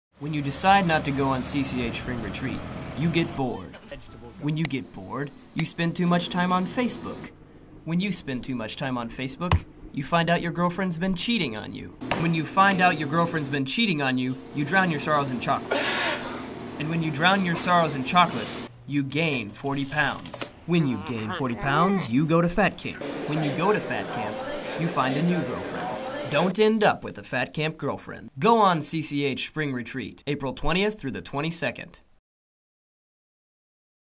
CCH Spring Retreat Commercial 2012
A commercial for the Mizzou Christian Campus House Spring Retreat 2012. It totally rocked our expectations.